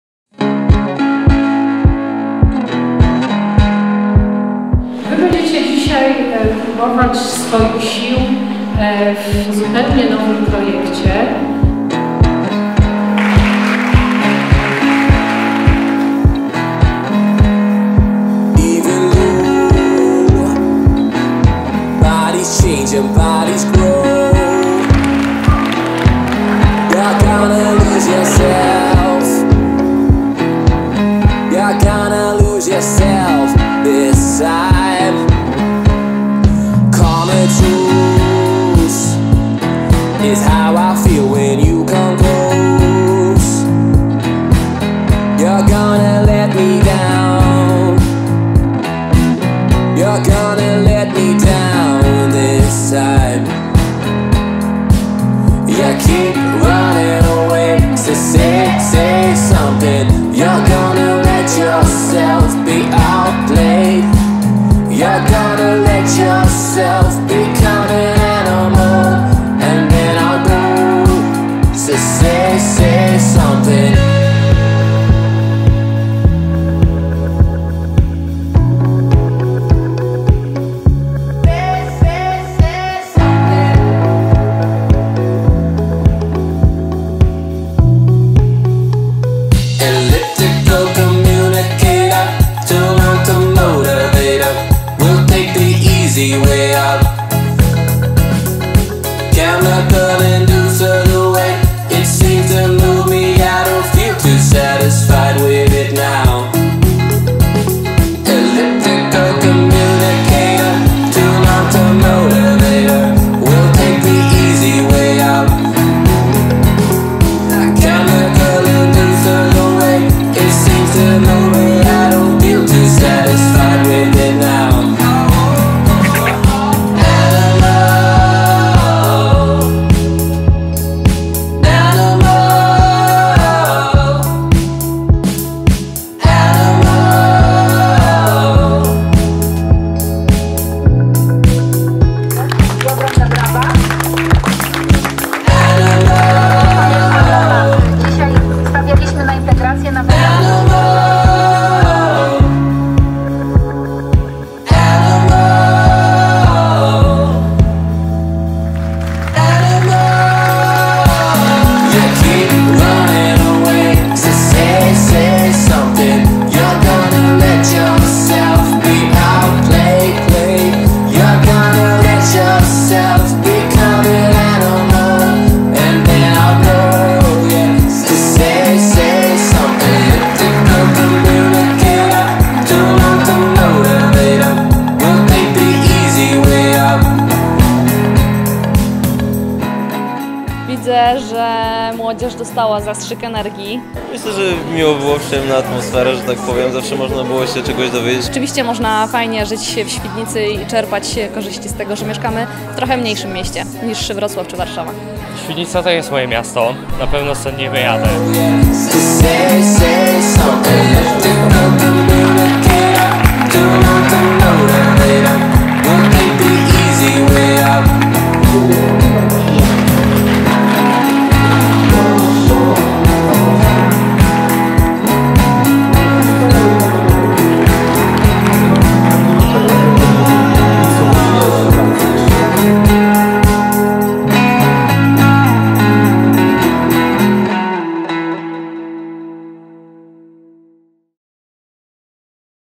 Zostańcie z nami 2018
"Zostańcie z nami - Miasto i Powiat Świdnica" to innowacyjny program gospodarczy ,który wyposaża młodzież w  wiedzę z zakresu kompetencji, pożądanych na lokalnym rynku pracy, wspiera ich w podejmowaniu życiowych wyzwań zawodowych, popularyzuje przedsiębiorczość i buduje postawy obywatelskie. W ciągu 3 lat 2500 uczniów świdnickich szkół poznało działalność lokalnych firm. Zorganizowano ponad 70 wizyt studyjnych w świdnickich przedsiębiorstwach, przeprowadzono ponad 180 zajęć z ekspertami doradztwa zawodowego i ekonomii. Stale rozwijany Program jest bardzo konkretną odpowiedzią na strategiczne pytanie - jaka przyszłość czeka miasta średnie w Polsce w dobie powszechnej depopulacji. W październiku 2019 roku program doceniony został, przez dolnośląski świat nauki i biznesu, elitarną nagrodą - Dolnośląskim Gryfem.